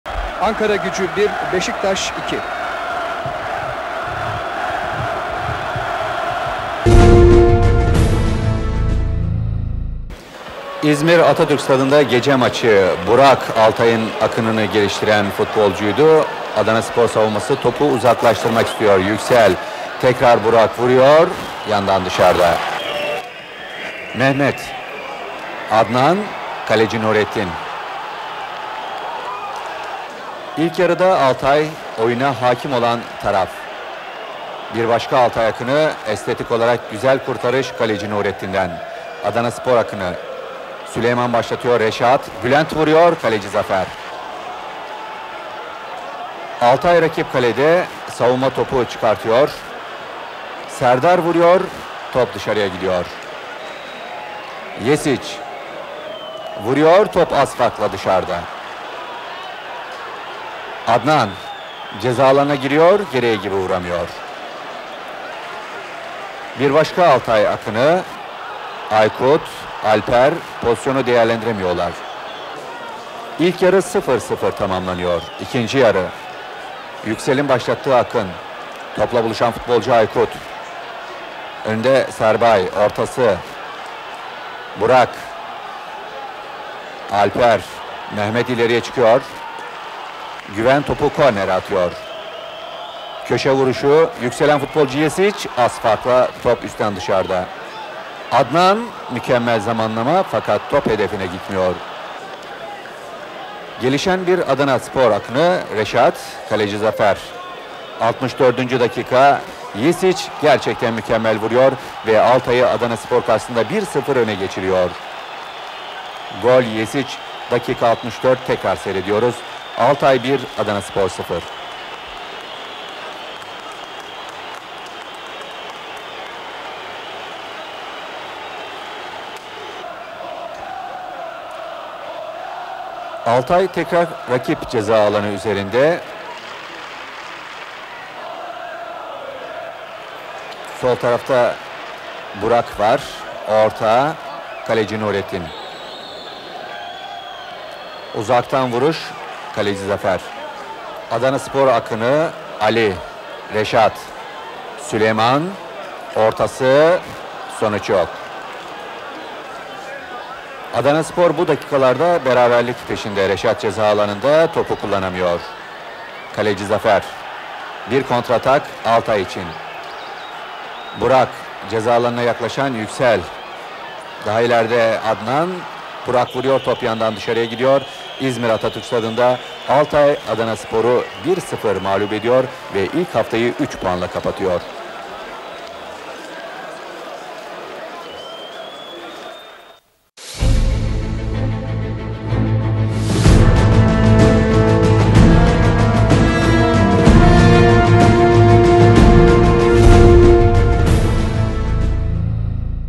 Nostalji - Özet | Altay-Adanaspor (1988-89) Yesiç'ten müthiş frikik golü
1989-90 sezonunda Altay kendi evinde Adanaspor'u 2-0 ile geçmeyi başardı. Altay'ın yıldızı Yesiç mükemmel bir frikik golüne imza attı.